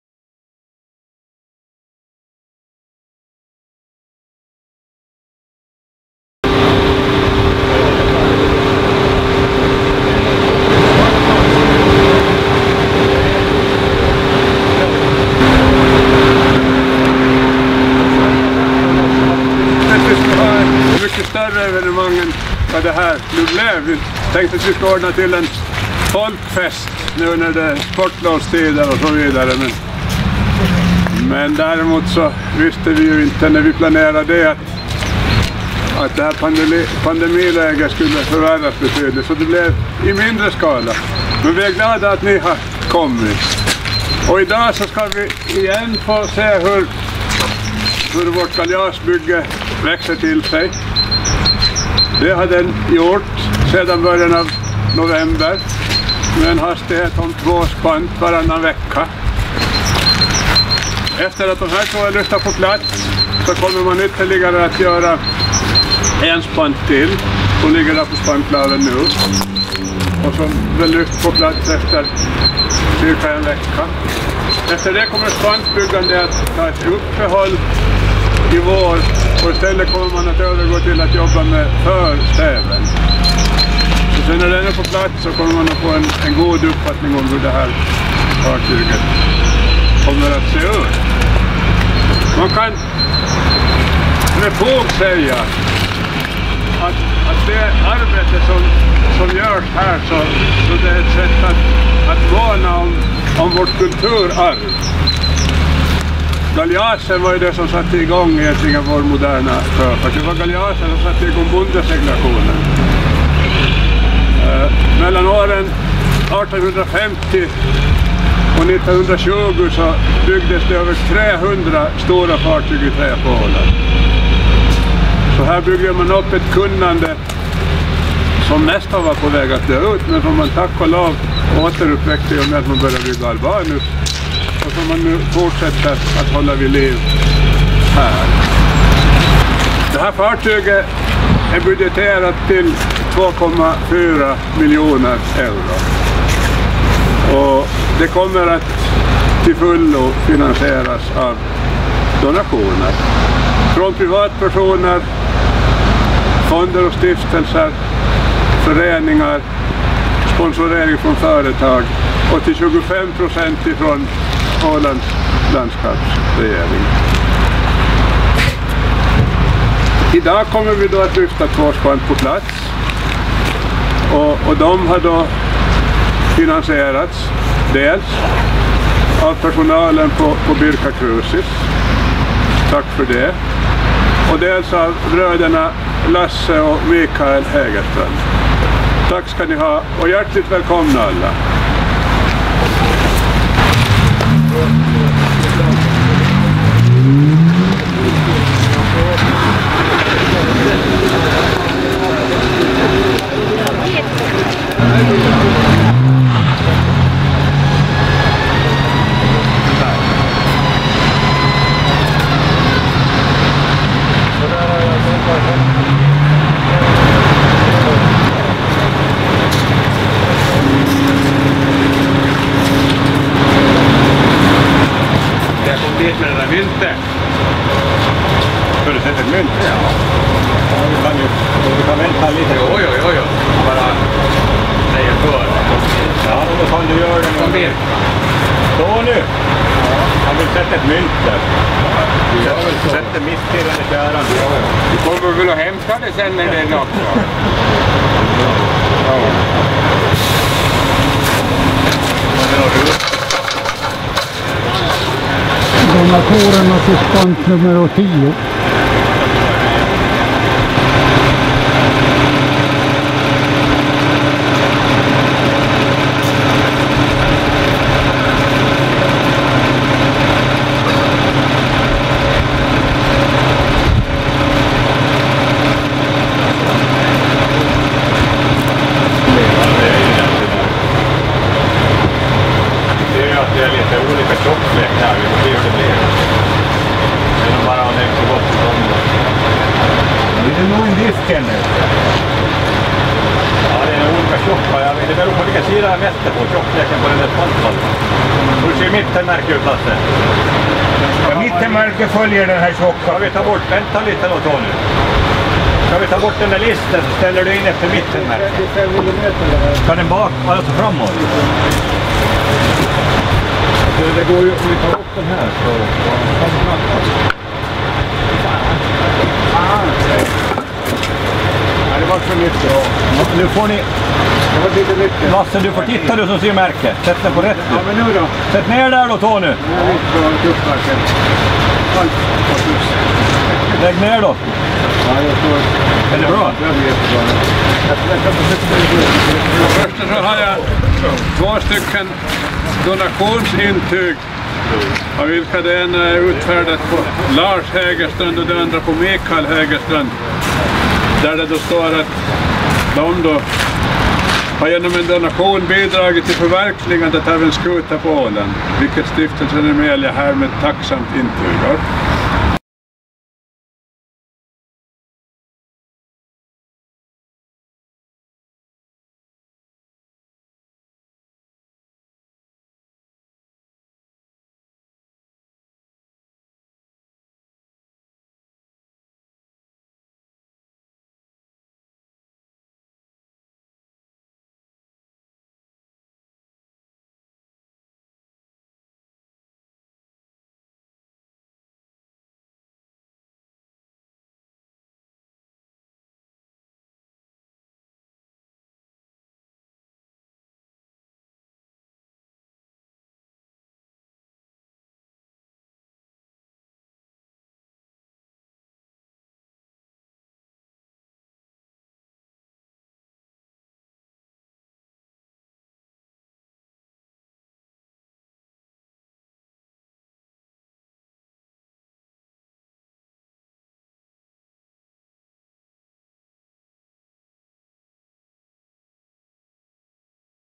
Emelia Åland, spant 11 och 10 monteras
Galeasen Emelia, Emeliabygget byggs i Sjökvarteret i Mariehamn på Åland.